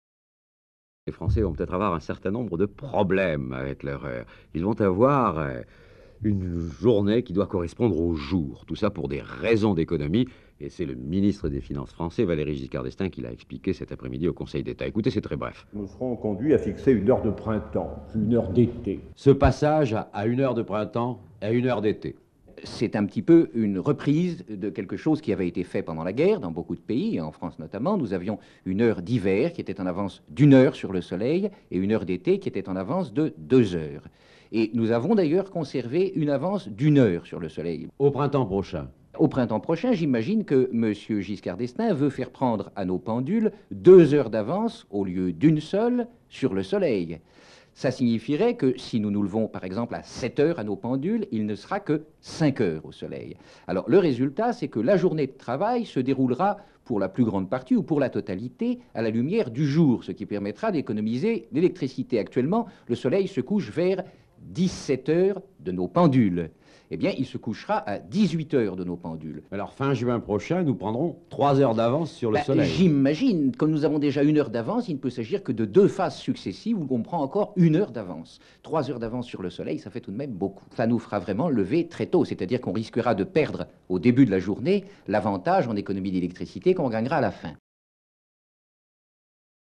Extrait archives M6 Video Bank // Le changement d'heure (RTL - Jacques Paoli -1973)
Retour sur la proposition du changement d'heure et du passage à une heure d'été par Valéry Giscard d’Estaing en 1973.

Toutes nos archives radio RTL sont disponibles à la vente pour les professionnels de l'audiovisuel et des médias.